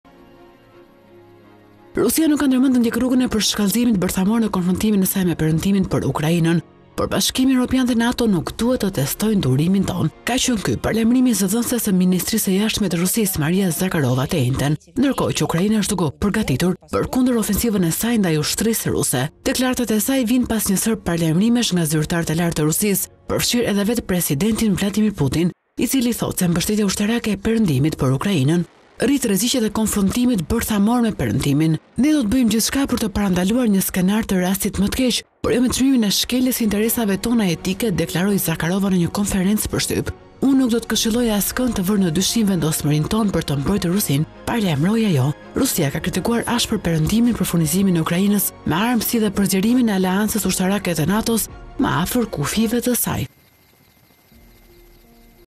Top News - Mos na testoni për armët bërthamore!/ Rusia paralajmëron NATO për furnizimin e Ukrainës
“Rusia nuk ka ndërmend të ndjekë rrugën e përshkallëzimit bërthamor në konfrontimin e saj me Perëndimin për Ukrainën, por BE dhe NATO nuk duhet të testojnë durimin e saj.”

Ka qënë ky paralajmërimi i zëdhënëses sëMinistrisë së Jashtme ruse Maria Zakharova të enjten ndërkohë që Ukraina po përgatitet për kundërofesivën e saj  kundër ushtrisë ruse.

Deklarata e saj vijnë pas një sërë paralajmërimesh nga zyrtarë të lartë rusë , përfshirë presidentin Vladimir Putin, se mbështetja ushtarake perëndimore për Ukrainën rrit rreziqet e një konflikti katastrofik bërthamor.

"Ne do të bëjmë gjithçka për të parandaluar një skenar të rastit më të keq, por jo me çmimin e shkeljes së interesave tona jetike," theksoi Zakharova në një konferencë për shtyp.

"Unë nuk do të këshilloja askënd që të vërë në dyshim vendosmërinë tonë," paralajmëroi ajo.

Rusia ka kritikuar ashpër Perëndimin për furnizimin e Ukrainës me armë dhe zgjerimin e aleancës ushtarake të NATO-s më afër kufijve të saj.

Pamje: AFP